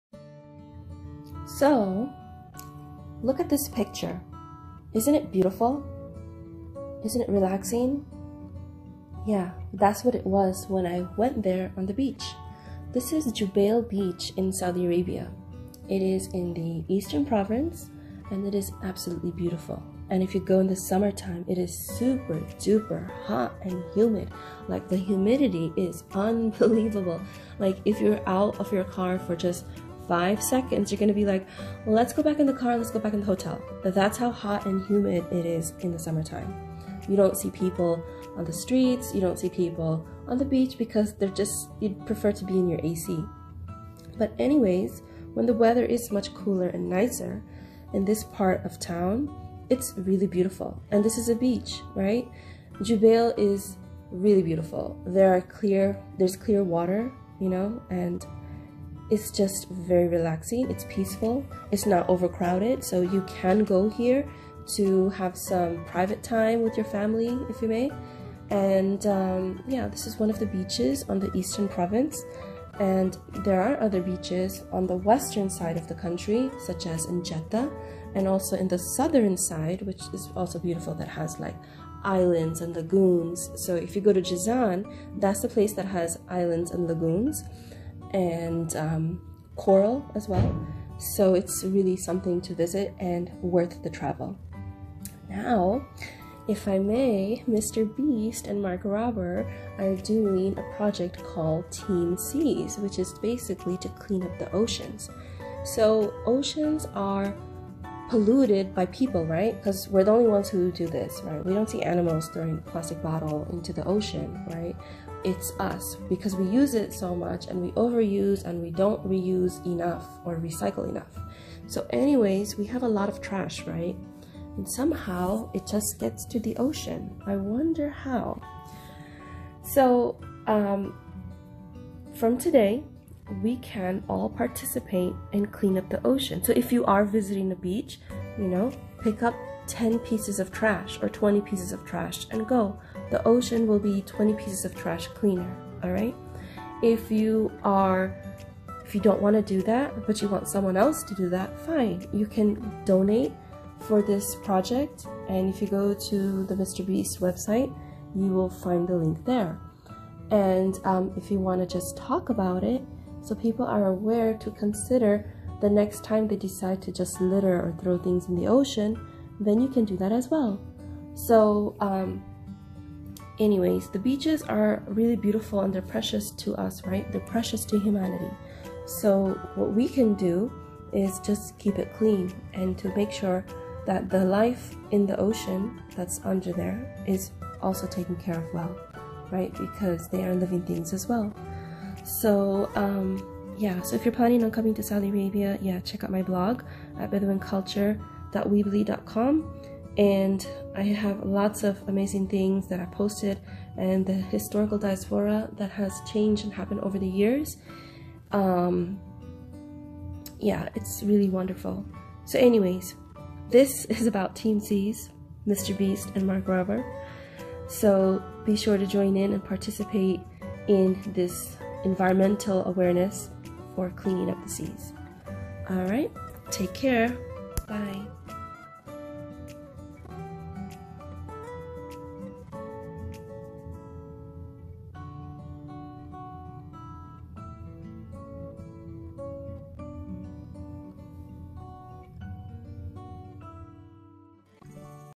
TeamSeas MrBeast and Mark Rober
TeamSeas with MrBeast and Mark Rober

In this video, I talk about the beaches of Saudi Arabia and why it is important to always keep the beaches clean. Join the challenge with MrBeast and Mark Rober to clean up the seas! Do your research and do your part to be able to enjoy these beautiful beaches FOREVER!

Wanna Learn Another Language? Purchase the Rosetta Stone by downloading it below
Rosetta Stone